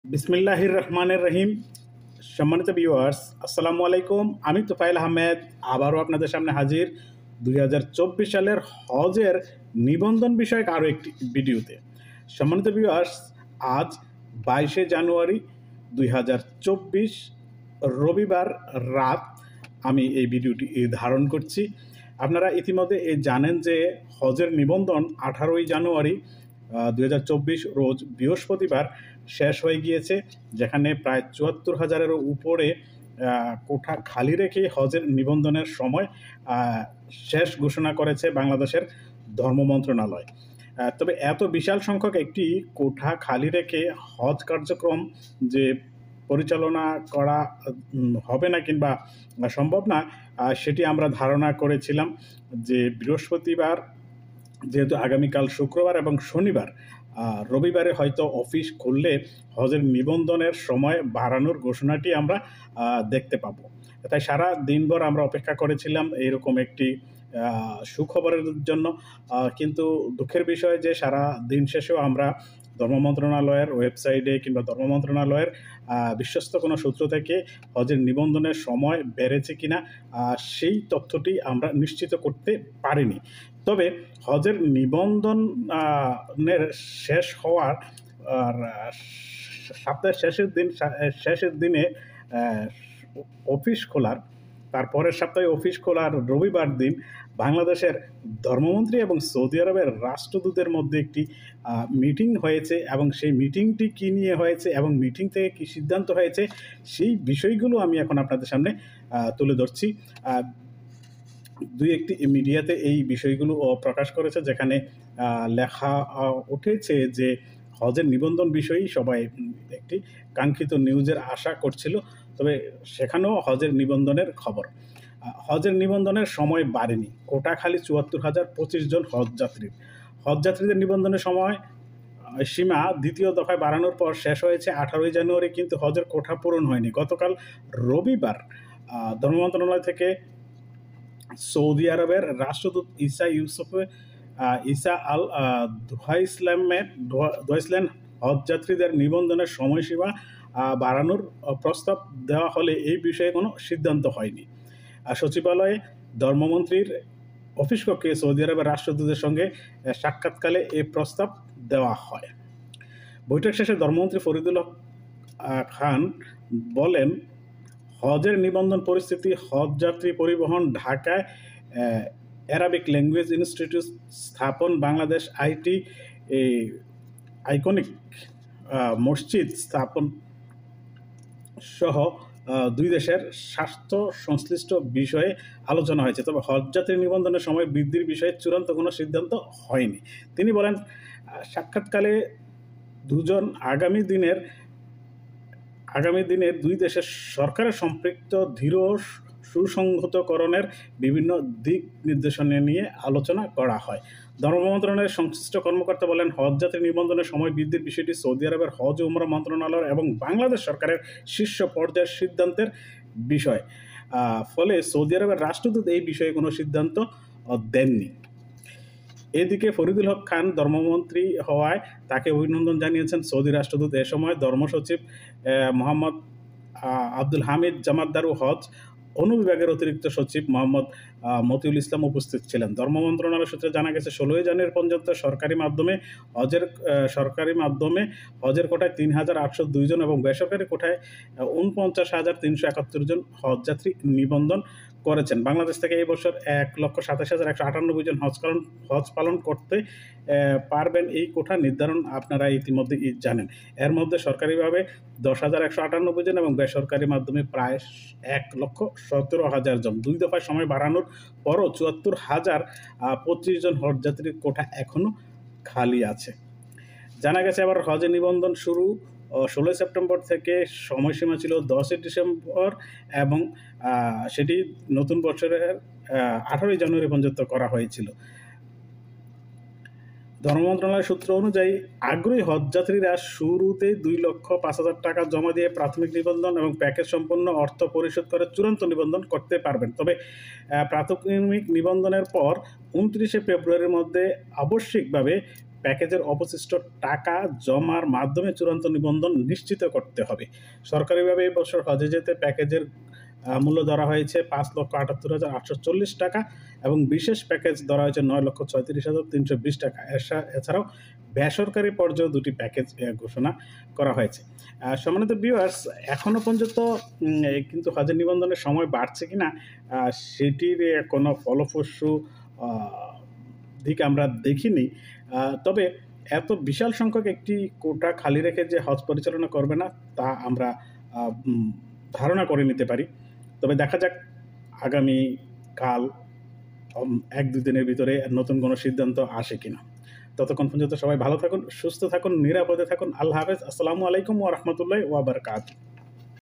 হজের নিবন্ধন সময় বাড়েনি | হজের চূড়ান্ত নিবন্ধনের সময় | নিবন্ধনের সময় শেষ | bd hajj registration
প্রকাশ: ২২ জানুয়ারি, ২০২৪ ০০:০০

হজের নিবন্ধনের সময় বাড়েনি

কোটা খালি ৭৪ হাজার ২৫ জন হজযাত্রীর

হজযাত্রীদের নিবন্ধনের সময়সীমা দ্বিতীয় দফায় বাড়ানোর পর শেষ হয়েছে গত ১৮ জানুয়ারি। কিন্তু হজের কোটা পূরণ হয়নি। গতকাল রবিবার ধর্ম মন্ত্রণালয় থেকে সৌদি আরবের রাষ্ট্রদূত ঈসা ইউসেফ ঈসা আল দুহাইলেনকে হজযাত্রীদের নিবন্ধনের সময়সীমা বাড়ানোর প্রস্তাব দেওয়া হলেও এ বিষয়ে কোনো সিদ্ধান্ত হয়নি। সচিবালয়ে ধর্মমন্ত্রীর অফিসকক্ষে সৌদি আরবের রাষ্ট্রদূতের সঙ্গে সাক্ষাৎকালে এ প্রস্তাব দেওয়া হয়।

বৈঠক শেষে ধর্মমন্ত্রী ফরিদুল হক খান বলেন, হজের নিবন্ধন পরিস্থিতি, হজযাত্রী পরিবহন, ঢাকায় এরাবিক ল্যাঙ্গুয়েজ ইনস্টিটিউট স্থাপন, বাংলাদেশে আটটি আইকনিক মসজিদ স্থাপনের বিষয়সহ দুই দেশের স্বার্থসংশ্লিষ্ট বিষয়গুলো নিয়ে আলোচনা হয়েছে। তবে হজযাত্রী নিবন্ধনের সময় বৃদ্ধির বিষয়ে চূড়ান্ত কোনো সিদ্ধান্ত হয়নি। তিনি বলেন, সাক্ষাৎকালে দুজন আগামী দিনে দুই দেশের মধ্যকার সম্পর্ক দৃঢ় ও সুসংহতকরণের বিভিন্ন দিক নিয়ে আলোচনা করেন।

ধর্ম মন্ত্রণালয়ের সংশ্লিষ্ট কর্মকর্তারা বলেন, হজযাত্রী নিবন্ধনের সময় বৃদ্ধির বিষয়টি সৌদি আরবের হজ ও ওমরাহ মন্ত্রণালয় এবং বাংলাদেশ সরকারের শীর্ষ পর্যায়ের সিদ্ধান্তের বিষয়।

ফলে সৌদি আরবের রাষ্ট্রদূত এ বিষয়ে কোনো সিদ্ধান্ত দেননি।

এদিকে ফরিদুল হক খান ধর্মমন্ত্রী হওয়ায় তাঁকে অভিনন্দন জানান সৌদি রাষ্ট্রদূত। এ সময় ধর্ম সচিব মু. আ. হামিদ জমাদ্দার ও হজ অনুবিভাগের অতিরিক্ত সচিব মো. মতিউল ইসলাম উপস্থিত ছিলেন।

ধর্ম মন্ত্রণালয় সূত্রে জানা গেছে, ১৬ জানুয়ারি পর্যন্ত সরকারি মাধ্যমের কোটায় তিন হাজার ৮০২ জন ও বেসরকারি কোটায় ৪৯ হাজার ৩৭১ জন হজযাত্রী নিবন্ধন করেছেন।

বাংলাদেশ থেকে এ বছর এক লাখ ২৭ হাজার ১৯৮ জন হজ করতে পারবেন। এর মধ্যে সরকারি মাধ্যমের কোটায় ১০ হাজার ১৯৮ জন ও বেসরকারি কোটায় এক লাখ ১৭ হাজার জন। দুই দফায় সময় বাড়ানোর পরও ৭৪ হাজার ২৫ জন হজযাত্রীর কোটা খালি আছে।

জানা গেছে, এবারের হজ নিবন্ধন শুরু হয় গত ১৬ সেপ্টেম্বর থেকে এবং সময়সীমা ছিল ১০ ডিসেম্বর পর্যন্ত। পরে ৩১ ডিসেম্বর পর্যন্ত সময় বাড়ানো হয়।

এরপর ১৮ জানুয়ারি পর্যন্ত নিবন্ধনের সময় বাড়িয়েছিল ধর্মবিষয়ক মন্ত্রণালয়।

ধর্ম মন্ত্রণালয়ের তথ্য অনুযায়ী, আগ্রহী হজযাত্রীরা শুরুতে দুই লাখ পাঁচ হাজার টাকা জমা দিয়ে প্রাথমিক নিবন্ধন এবং প্যাকেজের সম্পূর্ণ অর্থ পরিশোধ করে চূড়ান্ত নিবন্ধন করতে পারবেন। তবে প্রাথমিক নিবন্ধনের পর ২৯ ফেব্রুয়ারির মধ্যে আবশ্যিকভাবে প্যাকেজের অবশিষ্ট টাকা জমার মাধ্যমে চূড়ান্ত নিবন্ধন নিশ্চিত করতে হবে।

সরকারিভাবে এ বছর হজে যেতে সাধারণ প্যাকেজে পাঁচ লাখ ৭৮ হাজার ৮৪০ টাকা খরচ ধরা হয়েছে। বিশেষ প্যাকেজের মাধ্যমে হজ পালনে খরচ হবে ৯ লাখ ৩৬ হাজার ৩২০ টাকা। এ ছাড়া বেসরকারি পর্যায়ে দুটি হজ প্যাকেজ ঘোষণা করা